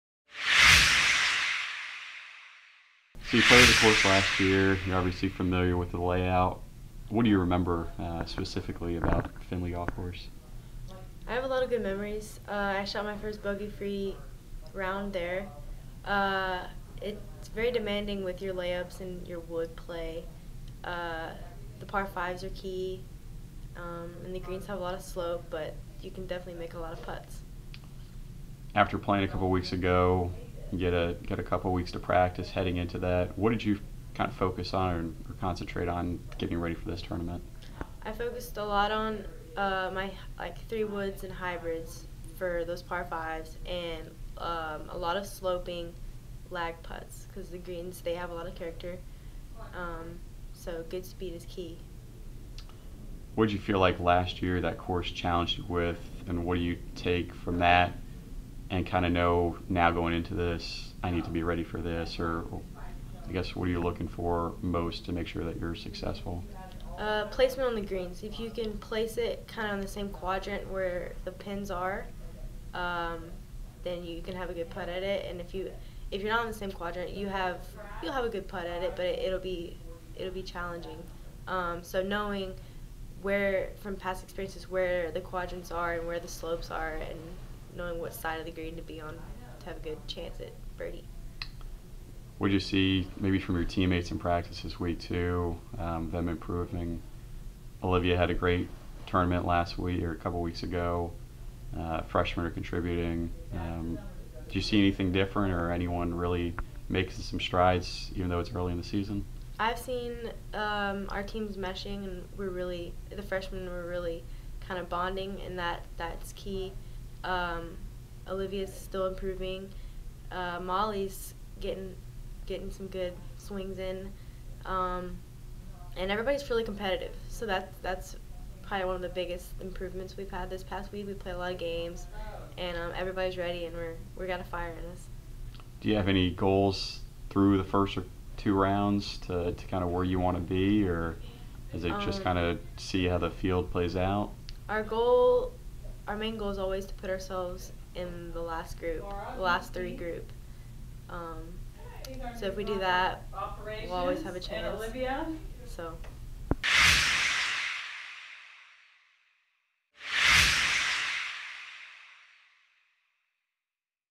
WGLF: Katie Mitchell Previews Tar Heel Invitational
Louisville women's golf senior Katie Mitchell spoke with CardsTV to preview the team's competition at the Tar Heel Invitational.